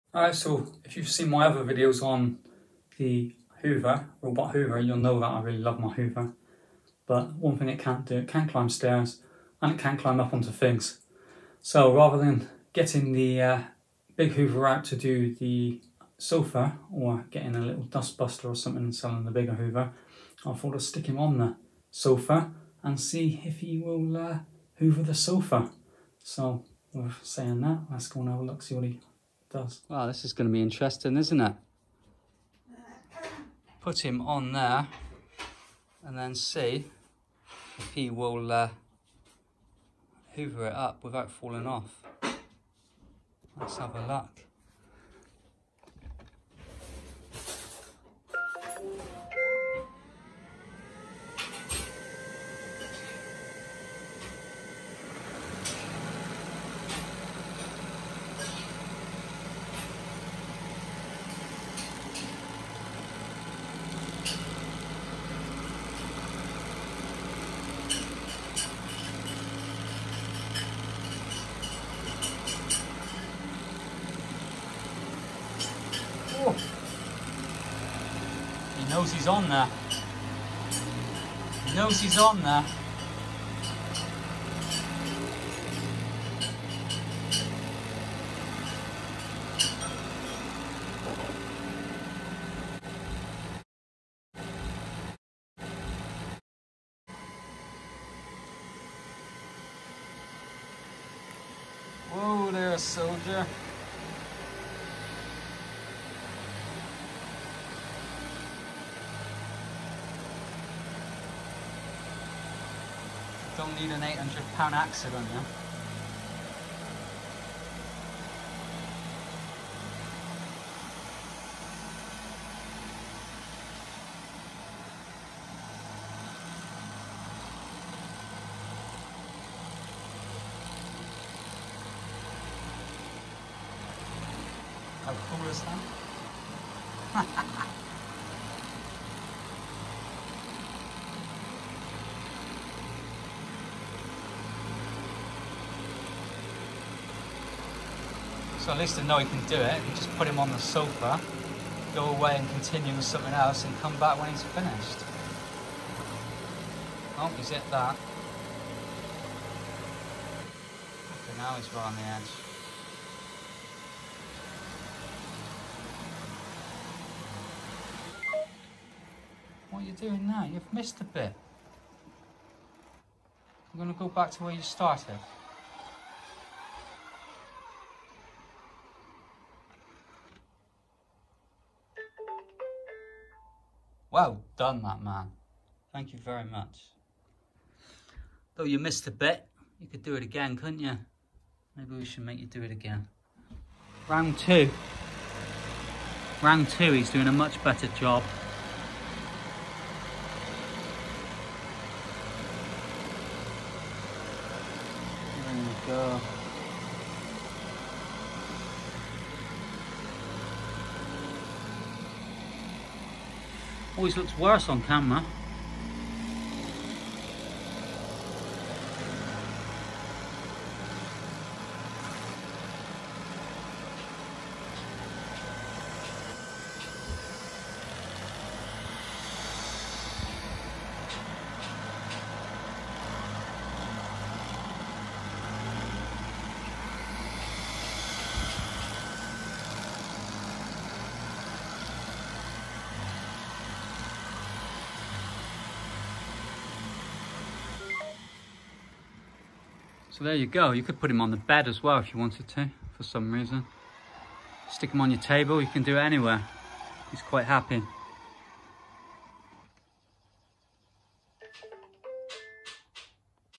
Neato Robot hoover falls off sofa?!
Can a robot hoover a sofa? Neato robotics bot vac. Neato robotics, D10, 9, 8, 7, 6, 5, 4, 3, 2, 1.